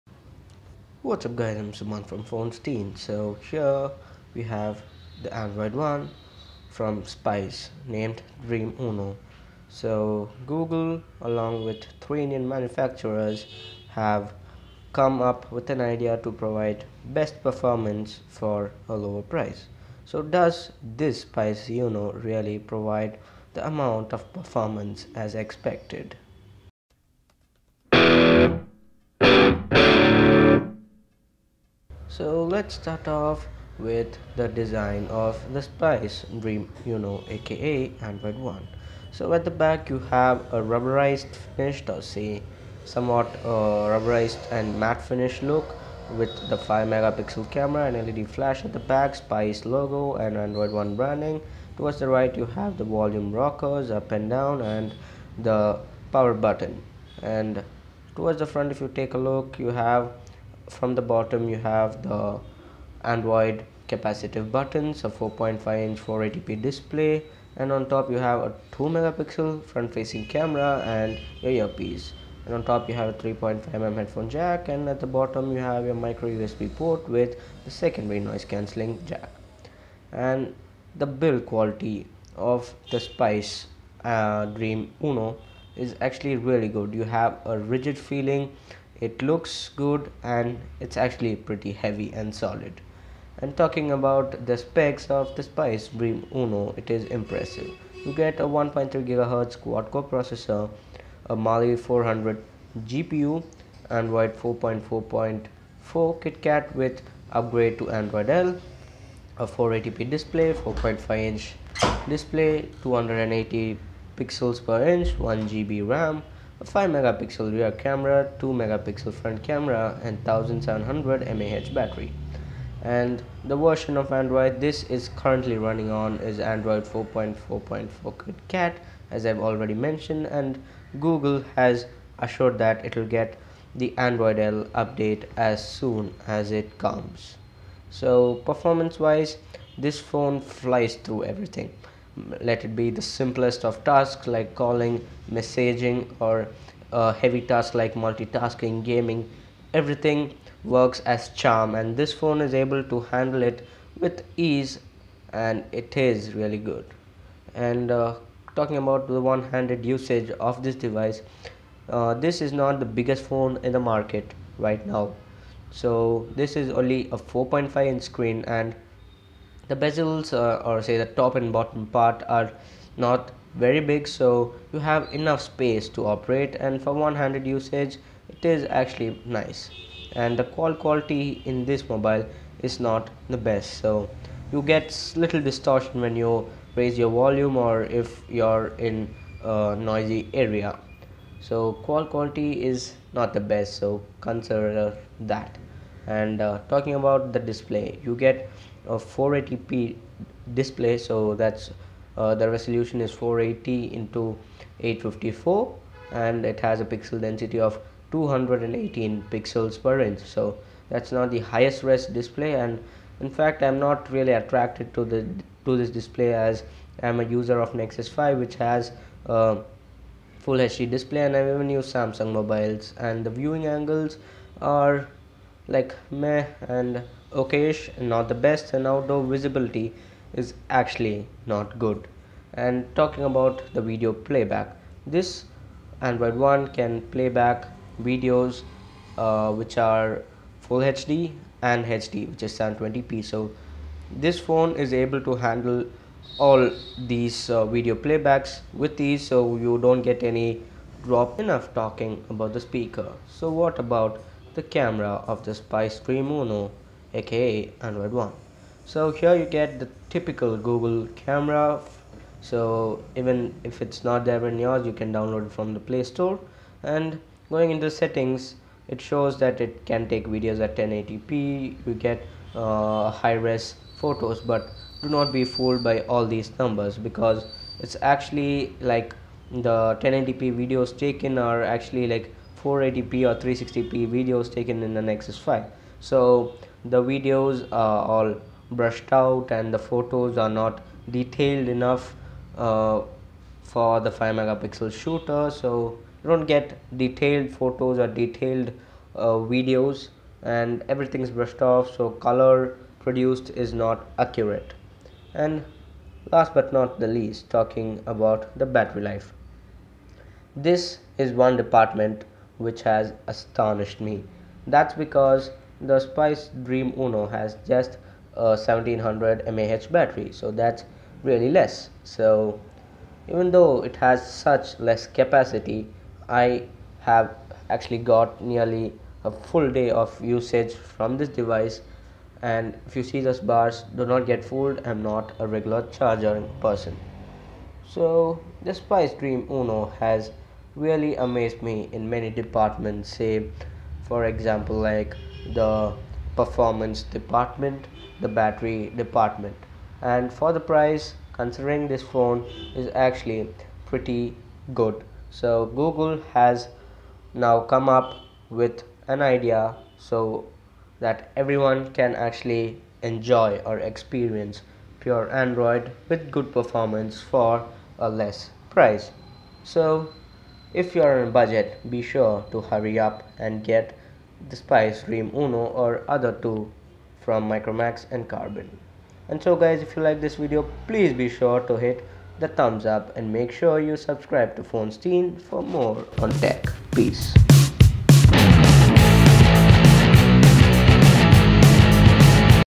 Android One ( Spice Dream Uno ) In-Depth Review !
The low-end high performer has now proved that google was right about them ! The Android One ( Spice Dream Uno ) has surely outdown all the nonsense i've thrown at it ! Here's my Indepth Review.